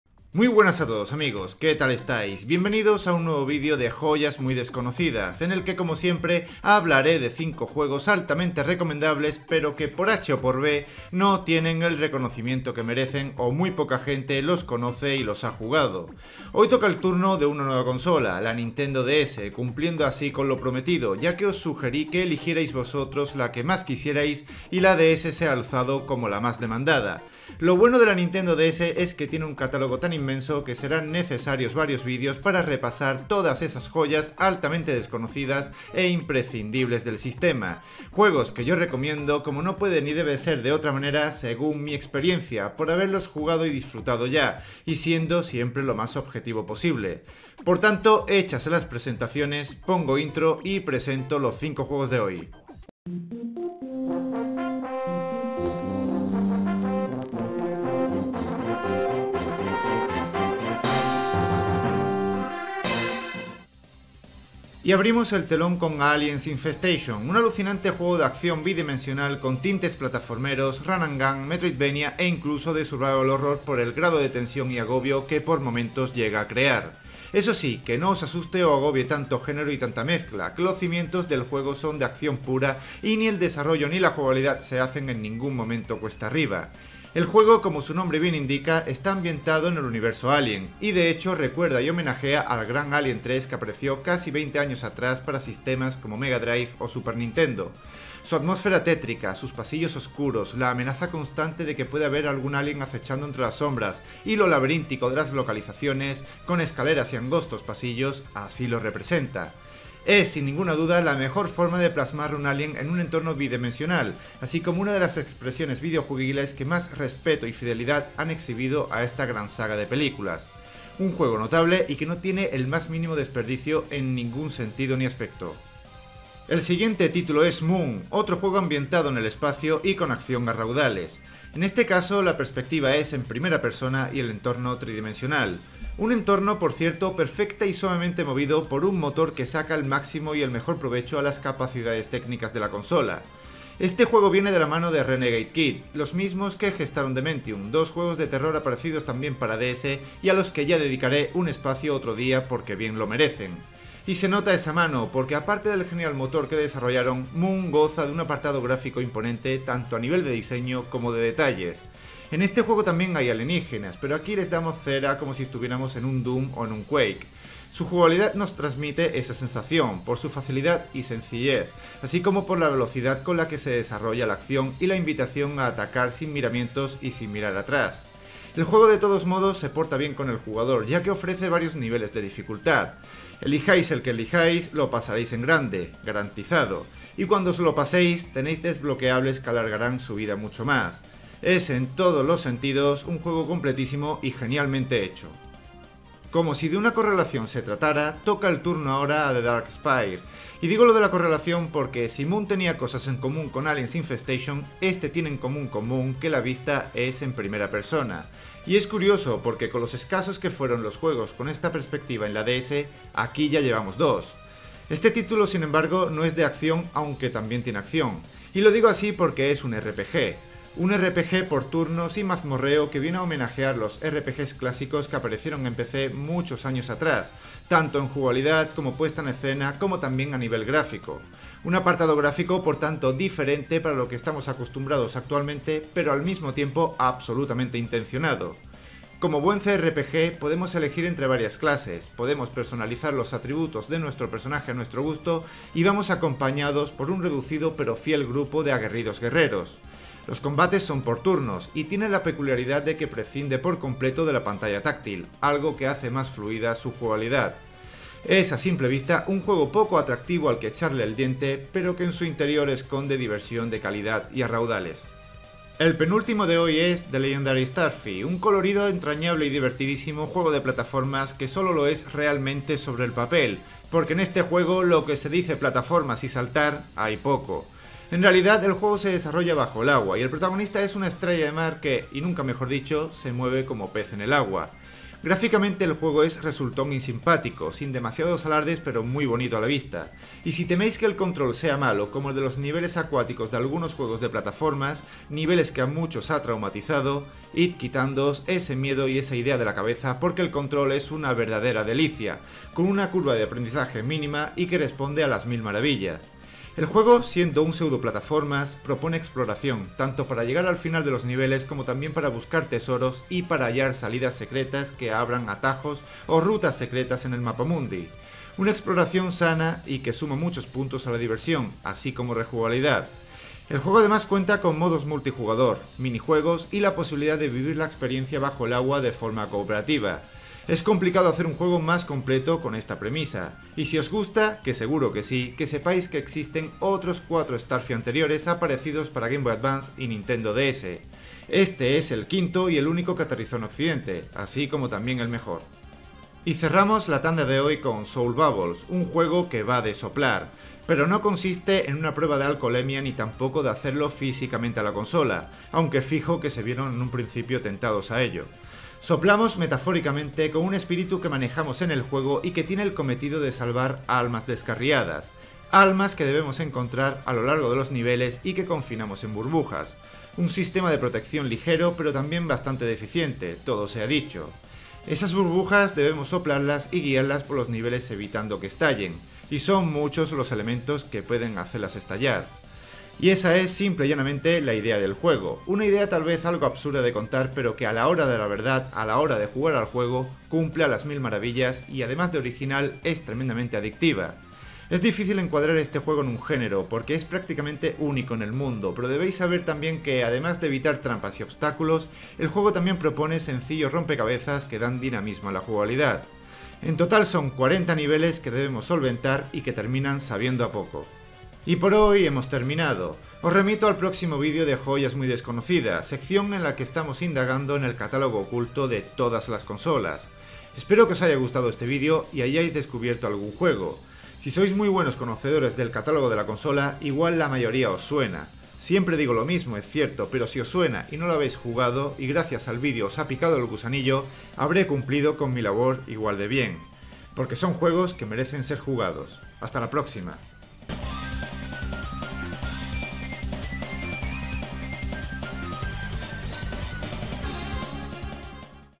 Joyas MUY desconocidas de Nintendo DS [Parte 1]
Un surtido de juegos aparecidos para Nintendo DS que son tremenda e incomprensiblemente desconocidos y que atesoran una grandísima calidad. Este no es el típico vídeo de joyas ocultas que se puede encontrar a montones en Youtube. Aquí ahondamos más y doy a conocer juegos que muy poca gente conoce.